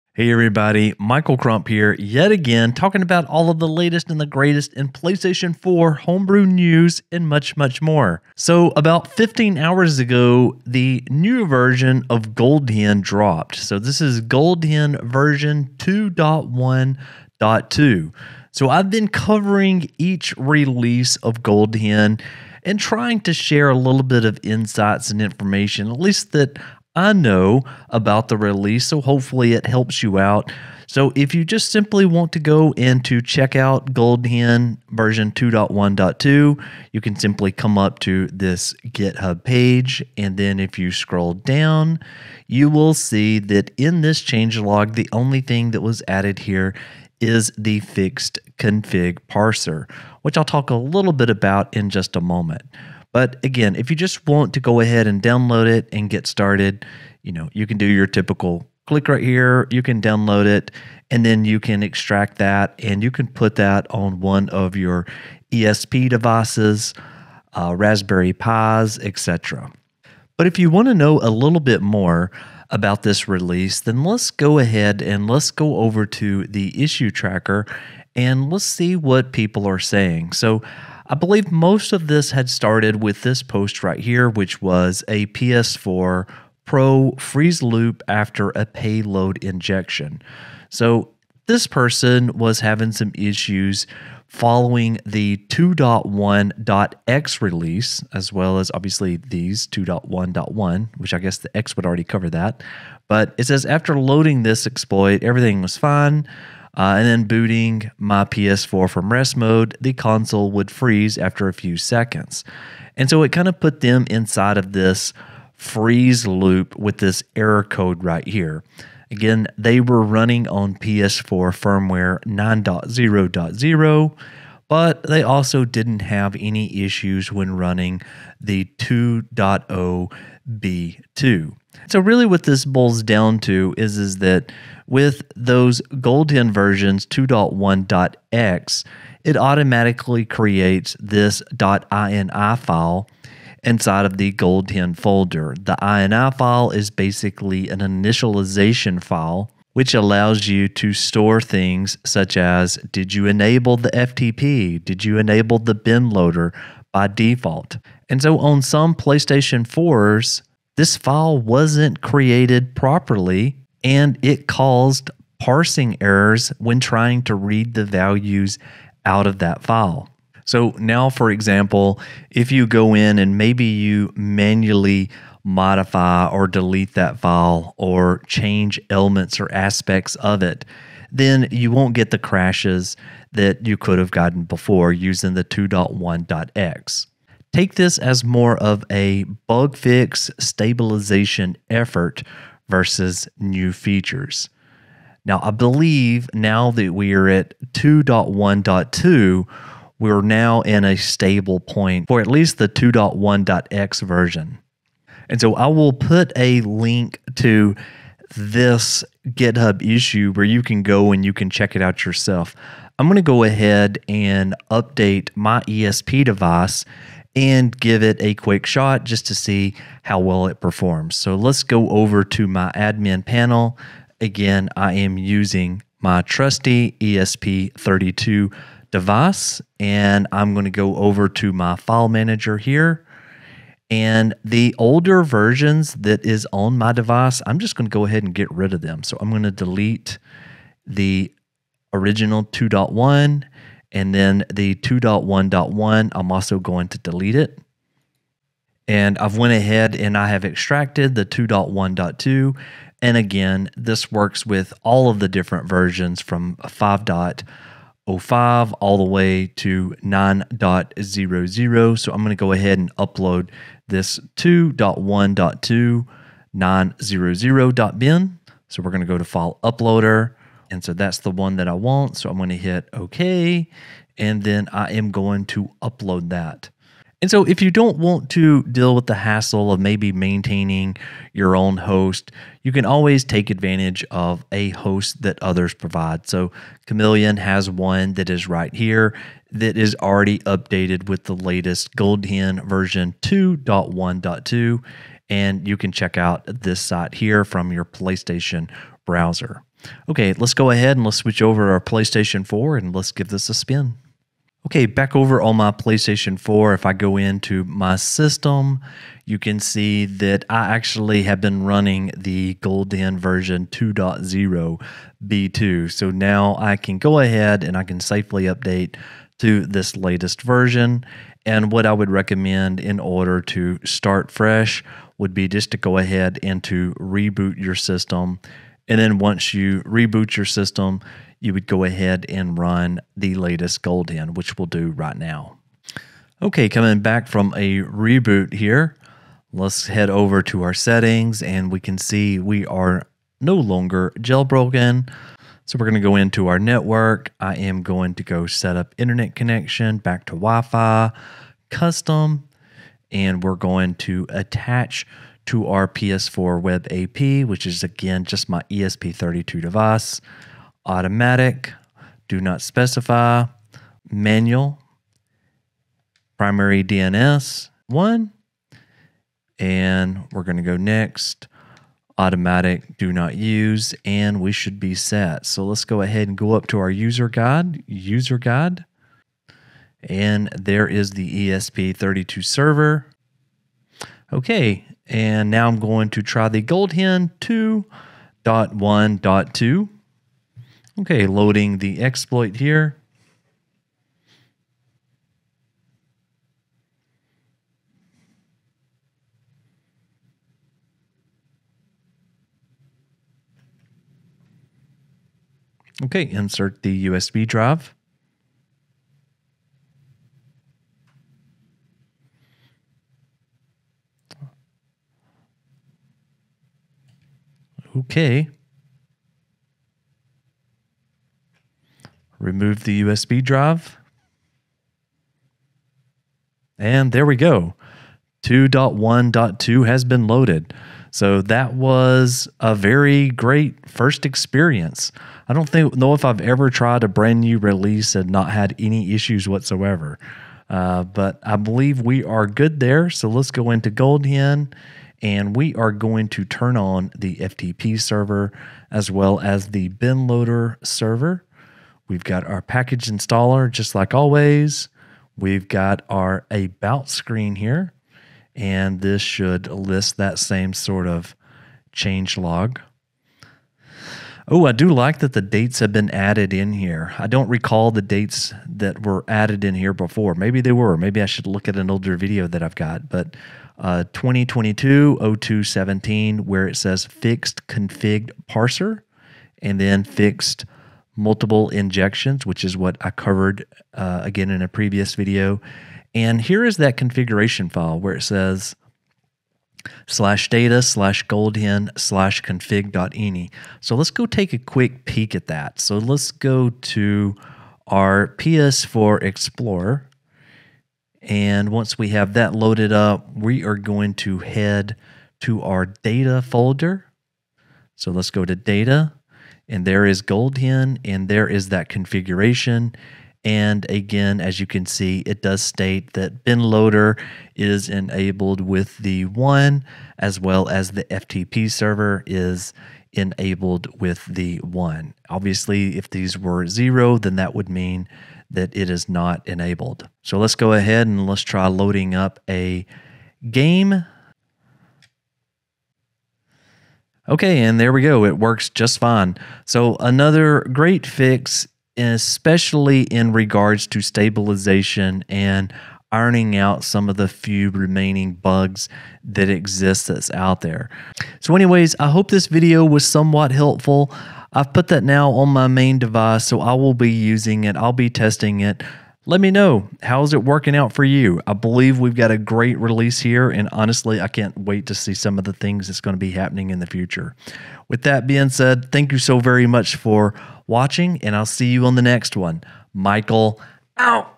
New GoldHEN v2.1.2 is out! What's New and should I update? YES! | PS4 Jailbreak
Release GoldHEN v2.1.2 · GoldHEN/GoldHEN

PS4 Pro Freeze Loop After Payload Injection · Issue #28 · GoldHEN/GoldHEN

(4) Kameleon on Twitter: "Hello Guys: My site has been updated with latest GoldHEN 2.1.2 Please delete cookies and Cache Data in order to get latest changes. Enjoy ;) / Twitter